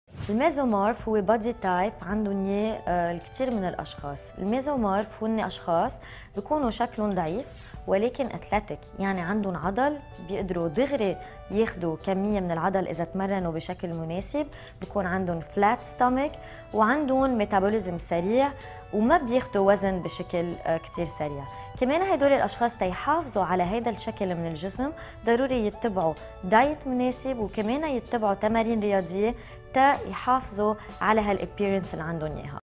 What Is A Mesomorph Body Shape? Dietitian Christelle Bedrossian, LBC, Beirut-Lebanon
In this one minute tip, Dietitian Christelle Bedrossian explains the mesomorph body shape.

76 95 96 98
Clinics in Adlieh and Jounieh
Lebanon